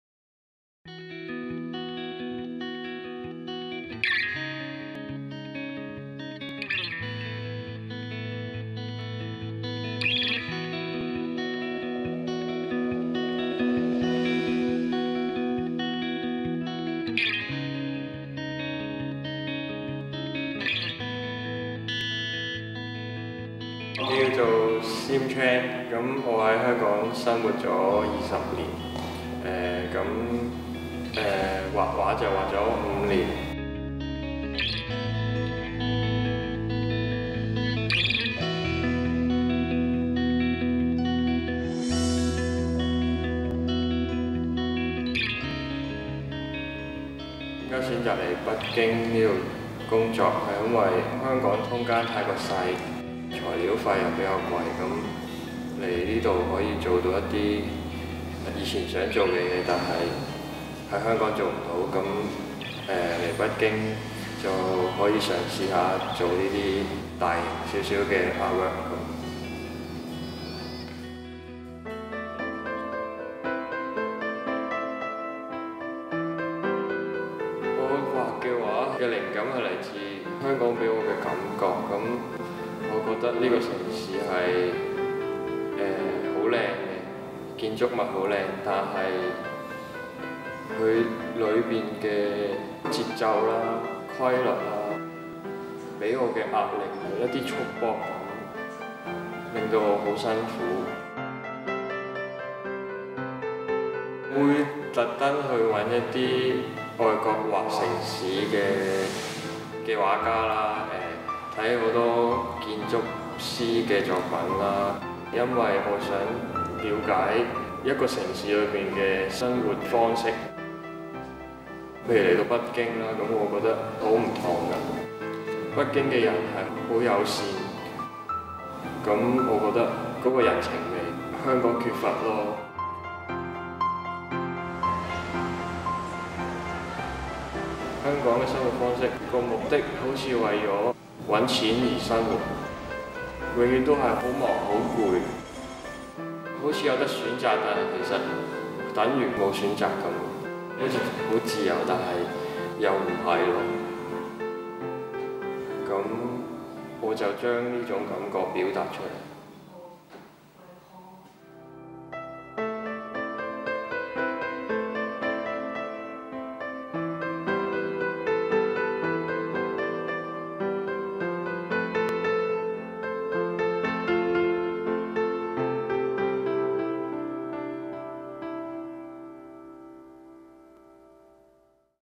Sim Chan on MischmaschTV
Young artist Sim Chan talks about his recent move from Hong Kong to Beijing.

Post production and editing: Hilda Chan
Interview conducted by: Alice Zhang
Special thanks to: Rita Chung